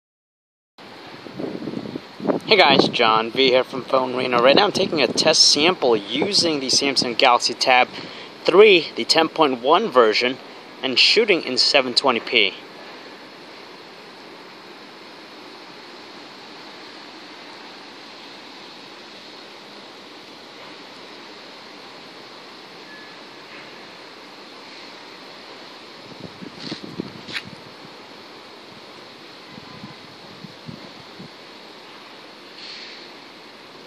Samsung Galaxy Tab 3 10.1 Sample Video
This is a sample video taken with Samsung Galaxy Tab 3 10.1.